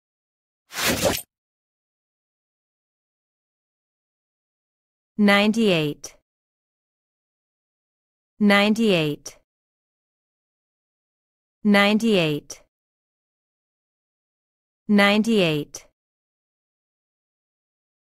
Ninety-eight (98) Pronunciation Correctly in English , How to Pronounce 98 in American English
Have you ever wondered how to pronounce 98 [ Ninety-eight ] in an American and a British accent? Or do you know someone who mispronounces it in the English language? Be sure to let them know about this video.